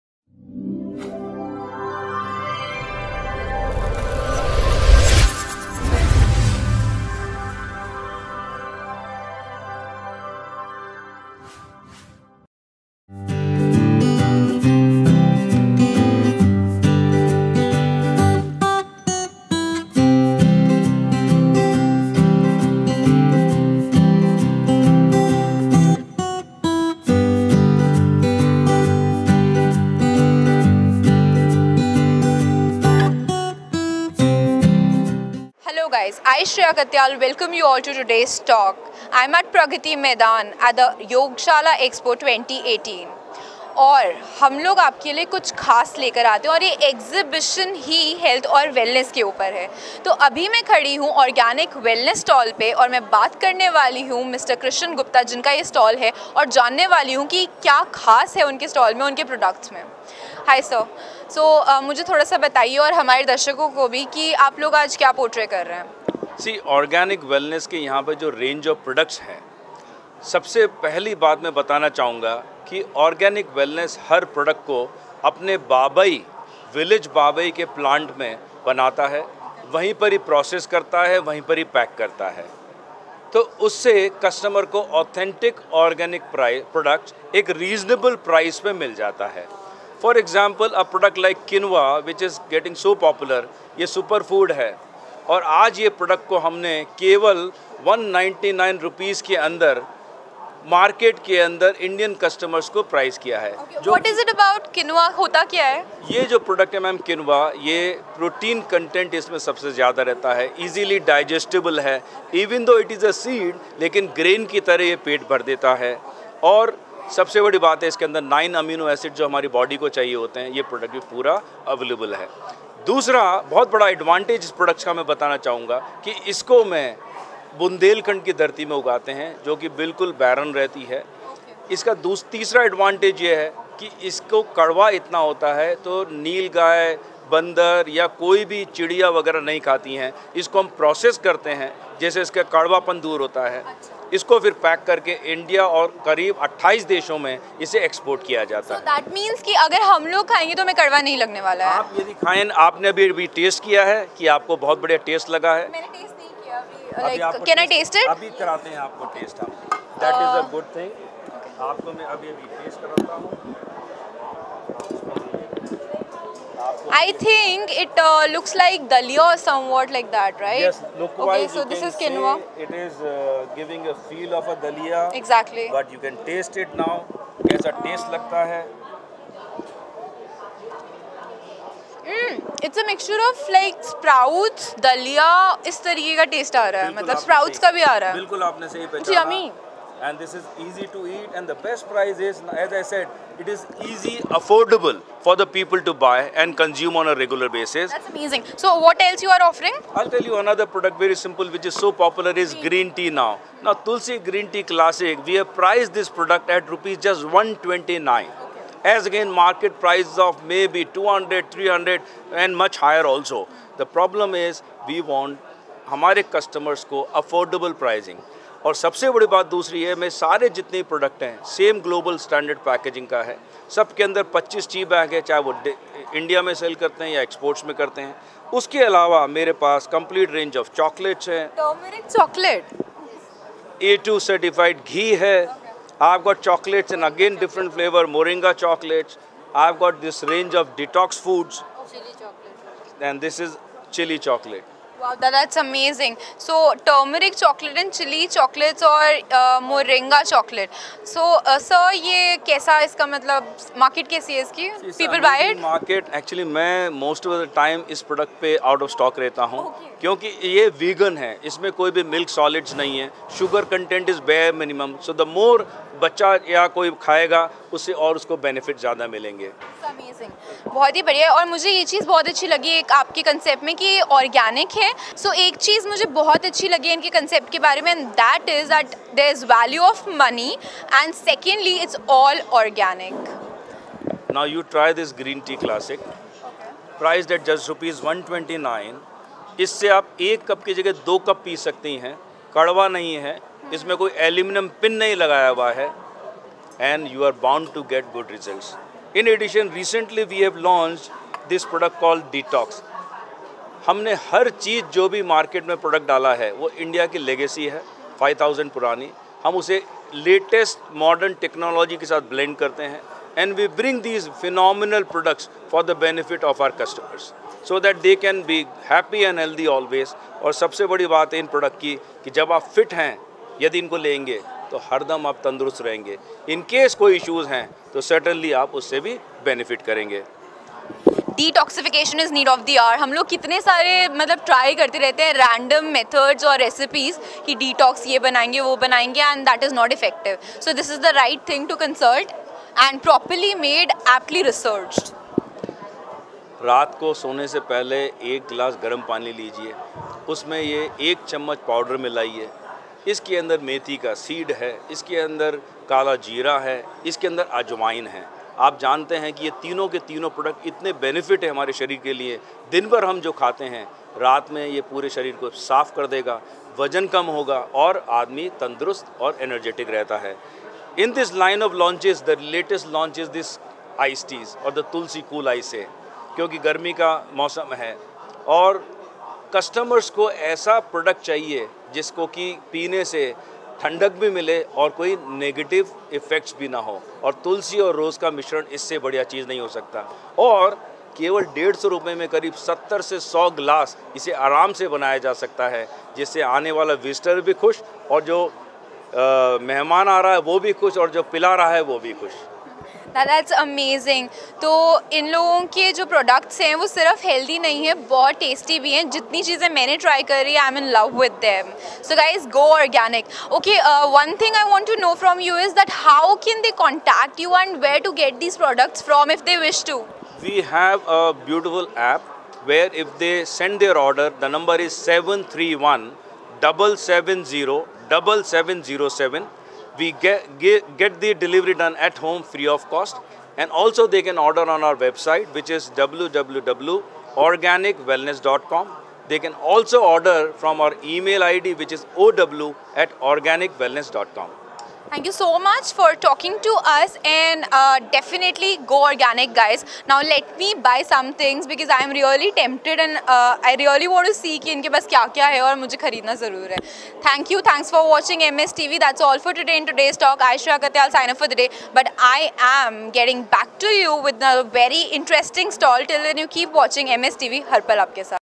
Organic Wellness Products Showcase in The Yogshala Expo - 2018
India’s Largest Health & Wellness Exhibition “The Yogshala Expo” invites all National & International Organic Food, Health & Wellness and Pharmaceutical Industries to Showcase Their Products at International Level each Year. Let’s Listen the words of Mr. Krishan Gupta, Organic Wellness.